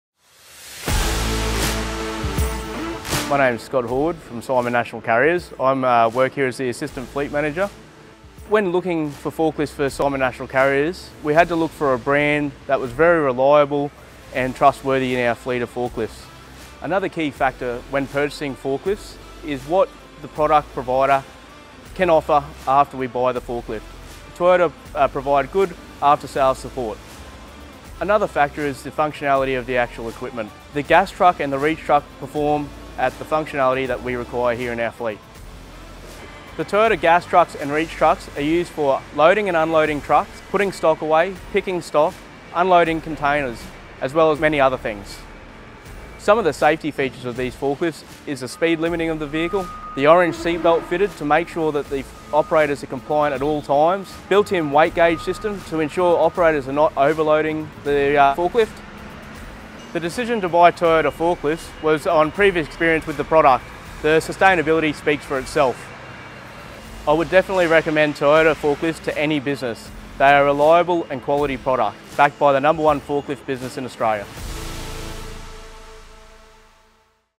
Toyota forklifts keep on trucking for Simon National Carriers
Australian trucking company puts trust in the exceptional safety and reliability of Toyota forklifts.

One of Australia's largest privately-owned transport companies, Simon National Carriers, has entrusted the exceptional safety, reliability and sustainability of Toyota Material Handling Australia (TMHA) to help keep its warehouses running at peak operational capacity.

Simon National Carriers recently purchased a fleet of 40 Toyota forklifts split between 7700 electric reach trucks and 32-8FG25 gas counterbalance forklifts, to be deployed among Simon's extensive array of depots across the country.